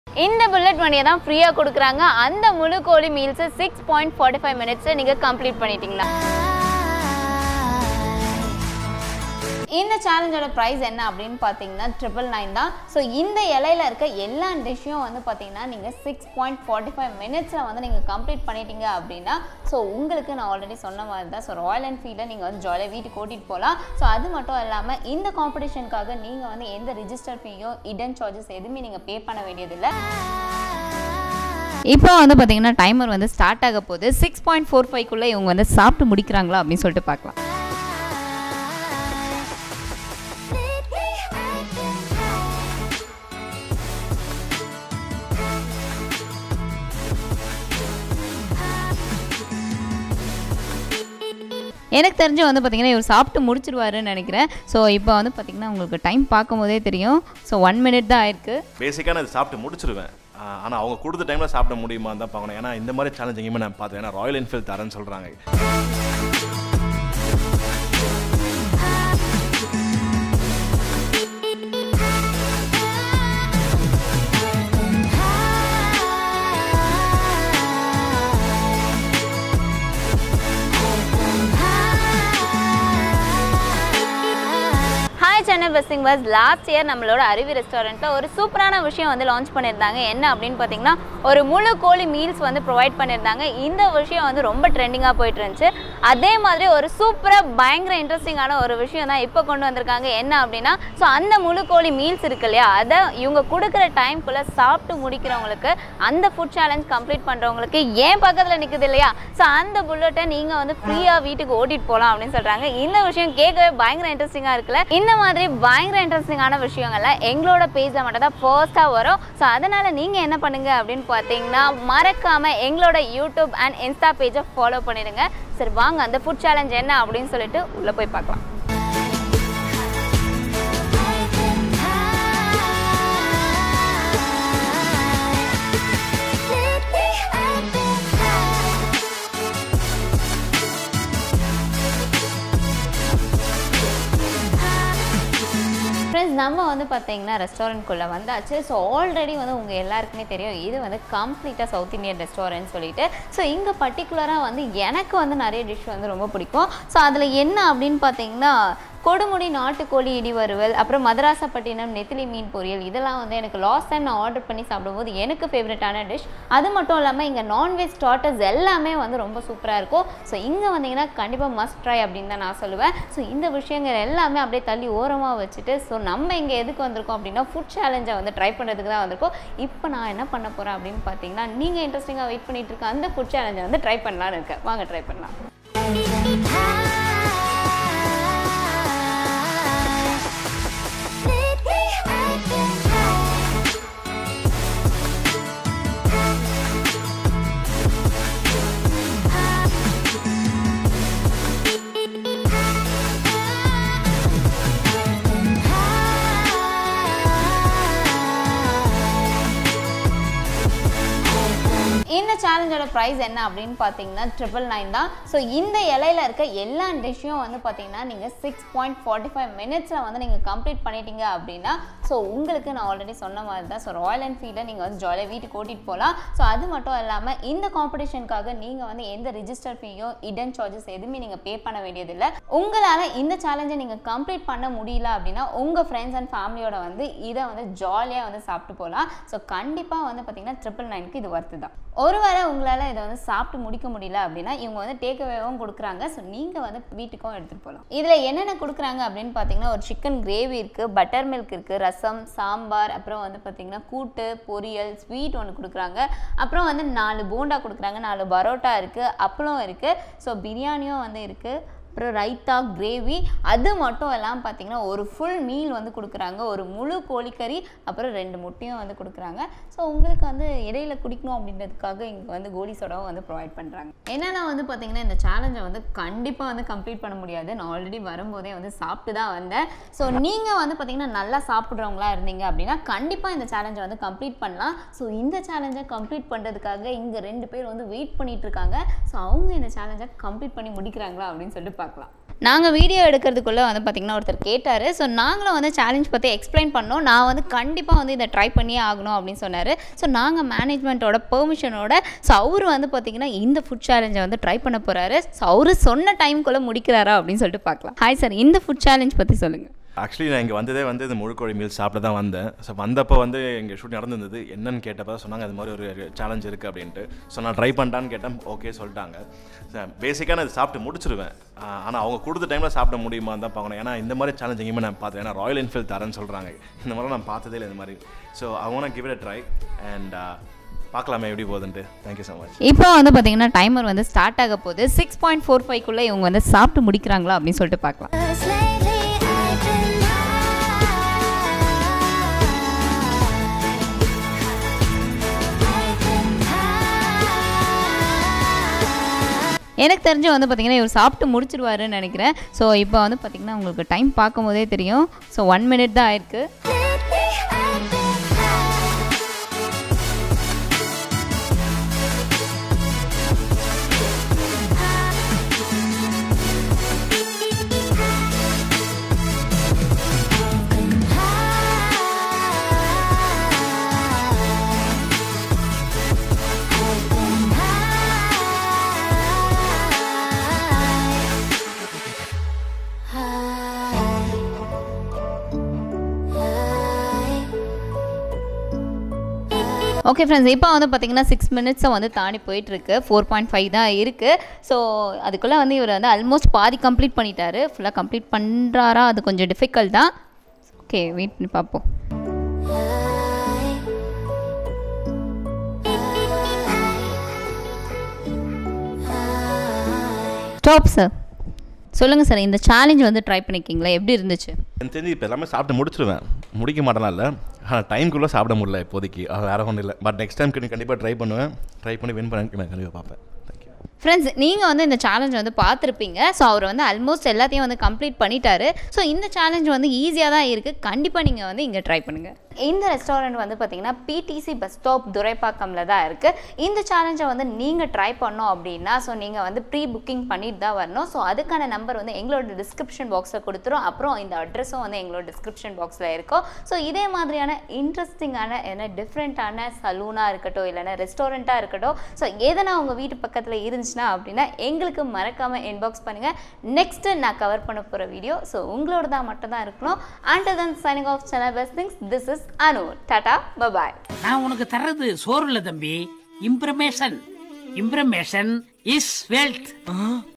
Biggest Food Challenge Ever || FREE ROYAL ENFIELD BIKE || Aravi Restaurant chennai || Food Review
Biggest Food Challenge Ever in Chennai

Eat And Win Royal Enfield Bike.

Finish a full Chicken meal in just 6 min 45 seconds to win an exclusive Royal Enfield bike, just for you. Unbelievable offer at Aruvi resturant ,grab yours soon.

Aruvi  Restaurant
Address 4/410 Venture Park First Floor ,Rajiv Gandhi Salai Thoraipakkam OMR Chennai  600097